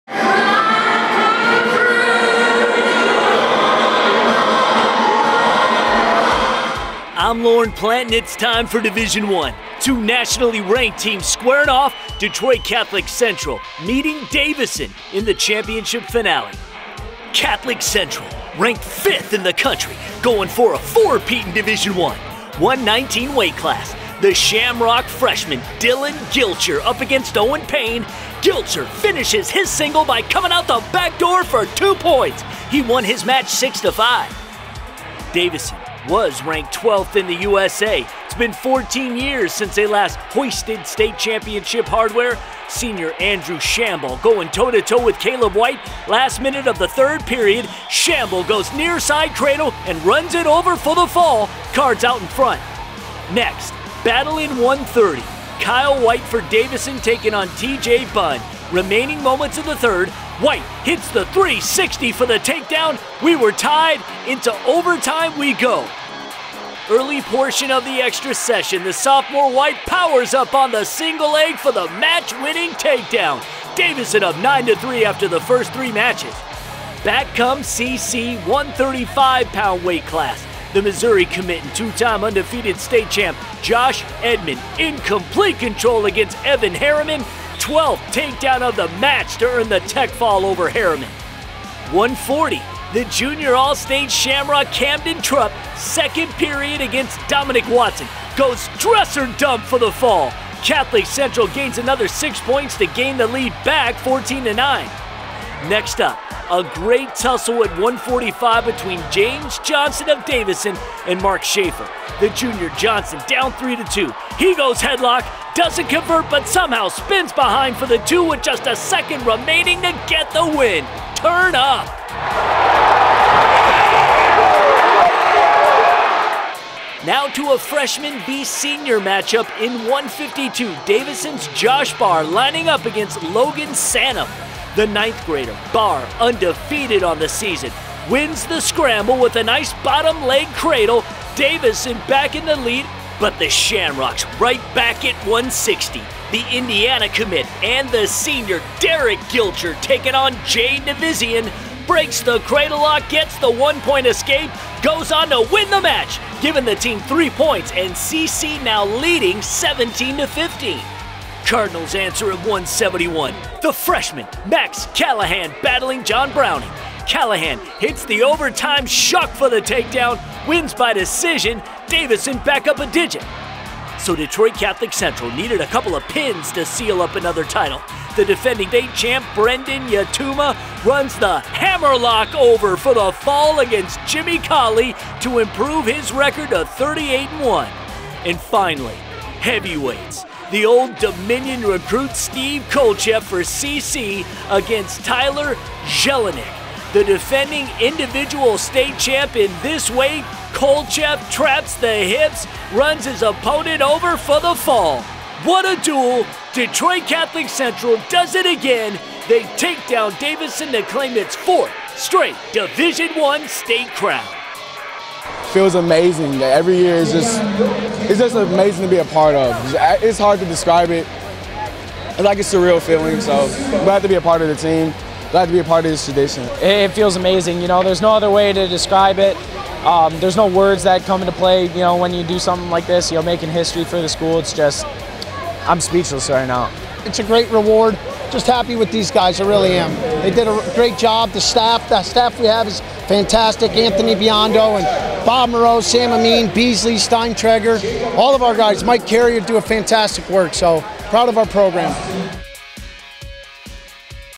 Detroit Catholic Central vs. Davison | 2020 Division 1 Team Wrestling Final
Check out the highlights of the Detroit Catholic Central High School vs. Davison - 2020 MHSAA Division 1 Team Wrestling State Final.